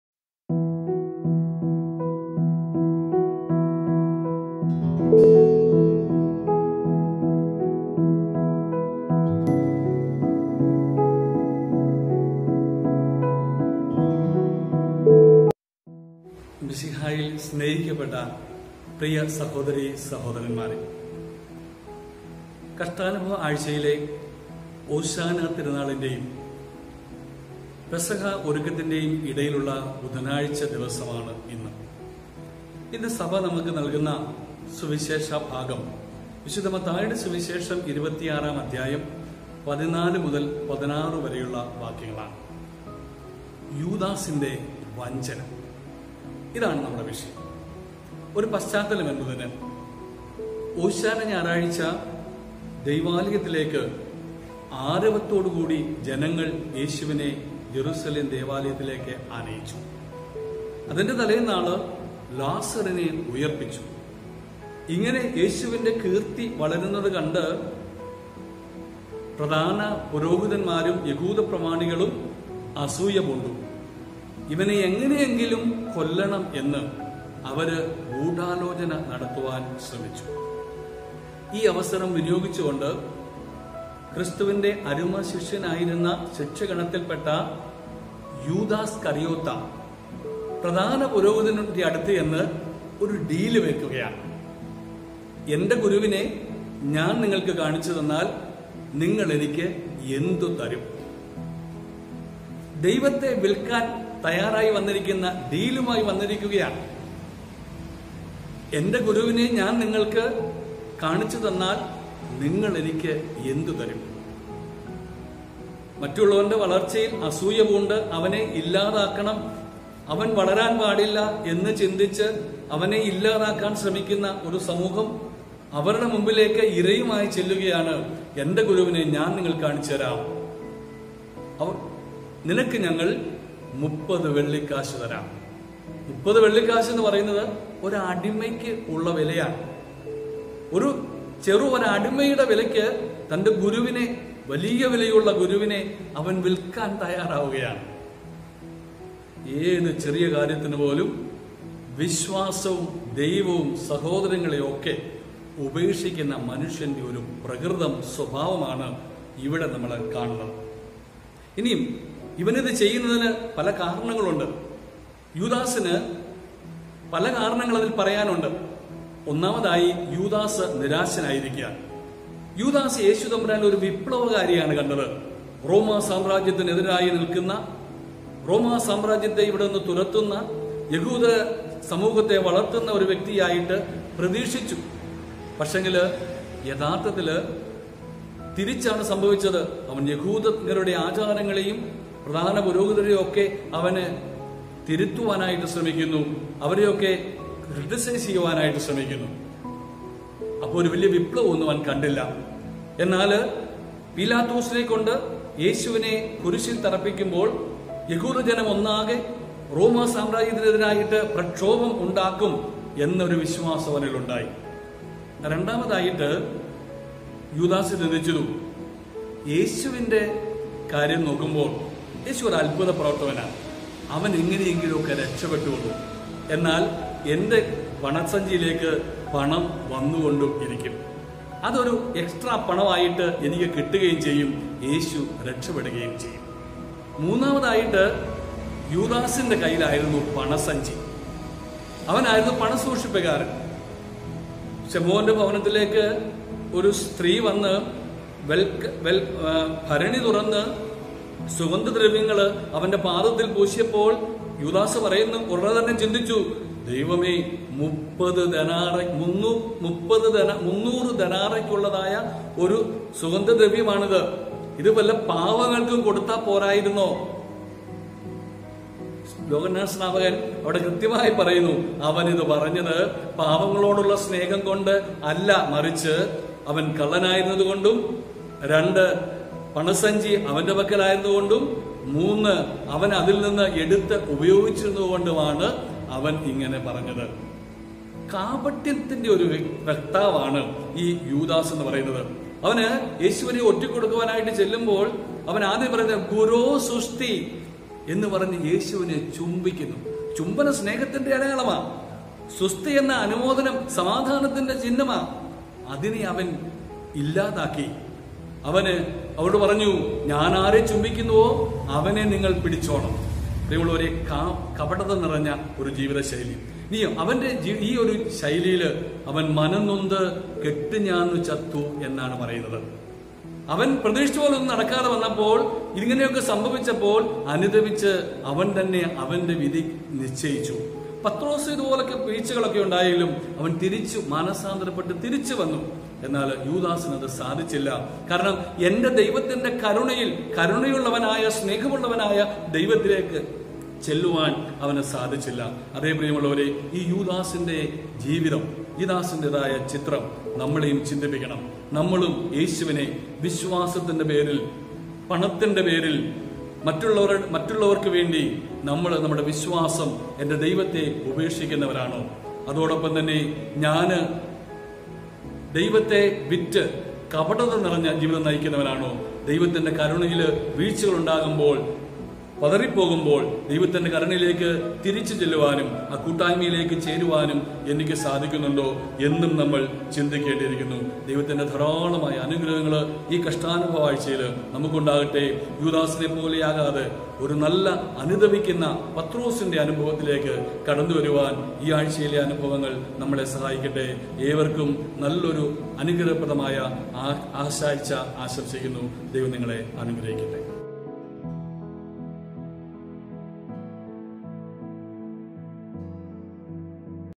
Reflection on Holy Lent Day 46 - Message by Fr. Thomaskutty Pathinettil
Message by Fr. Thomaskutty Pathinettil

Major Archdiocese of Thiruvalla

Malankara Syrian Catholic Church